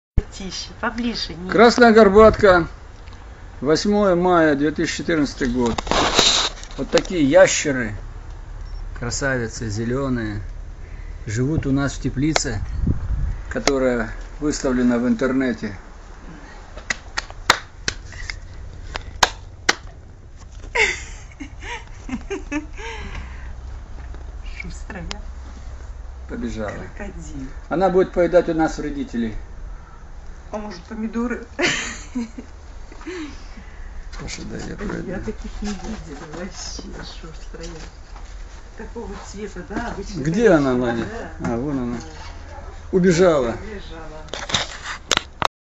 Ящер Красной Горбатки
В моей теплице поселился зелёный ЯЩЕР. Живёт он под крышей  рубероида. Очень хочу сделать фильм про его семью.
Продолжение будет!

Могу помочь ИСПРАВИТЬ зрение.